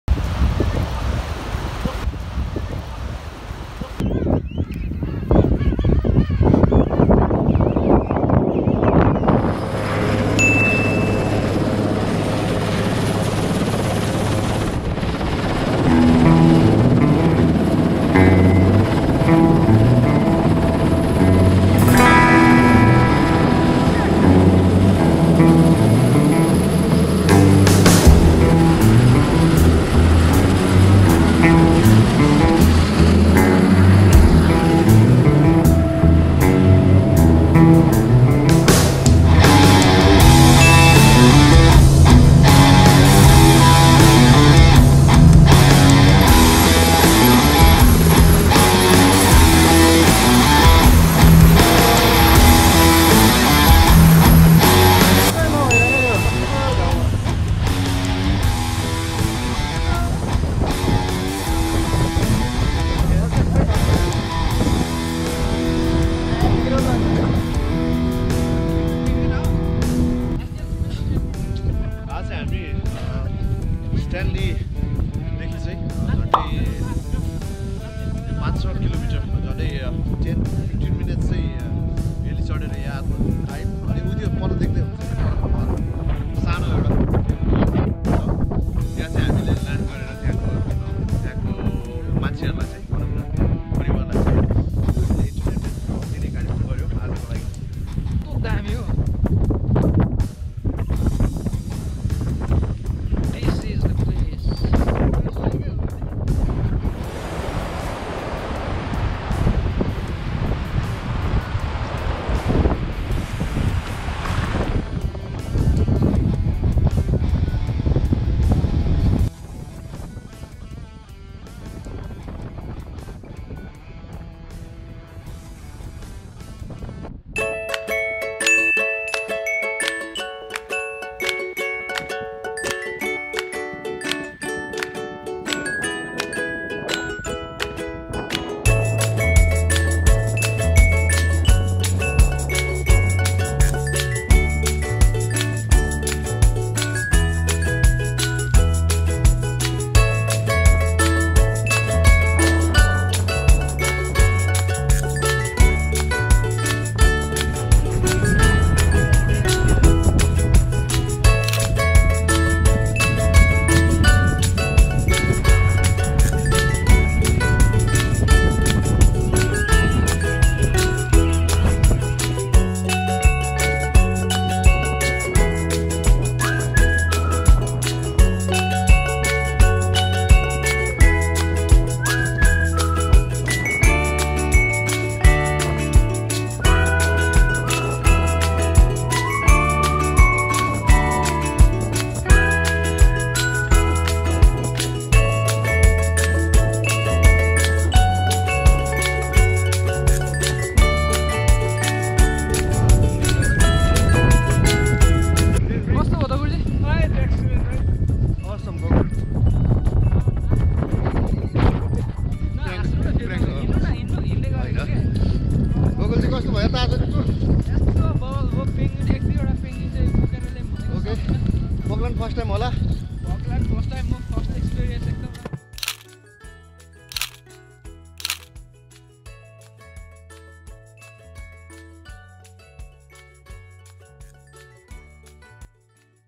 Falkland Island Tour2019||King Penguins Island-Travel Music and Life
It was my first tour to Falkland Island and will probably be the longest journey I will ever make in my life time.The distance from UK to Falkland Island is approx 8064miles and will take approx 22hrs in a flight. I managed to had a great time having a adventurous and practical tour though there is poor access of internet.